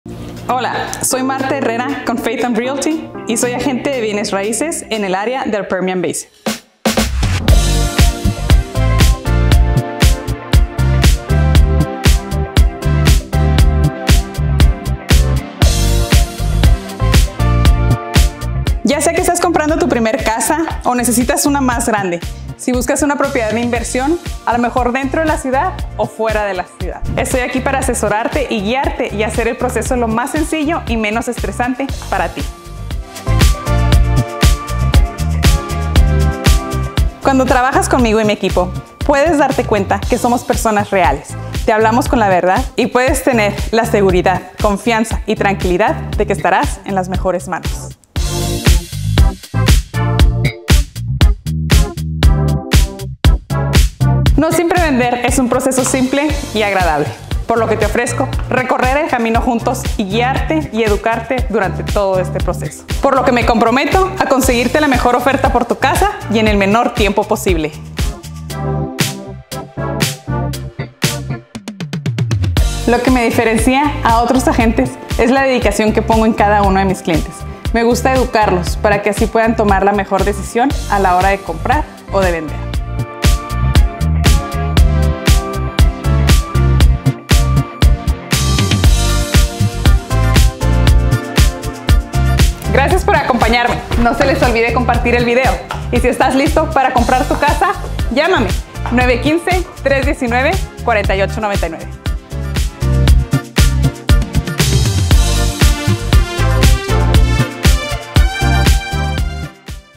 Martha Herrera Commercial - Real Estate Agent
Professional Real Estate Agent Martha Herrera takes the stage with her one of a kind commercial shot by yours truly.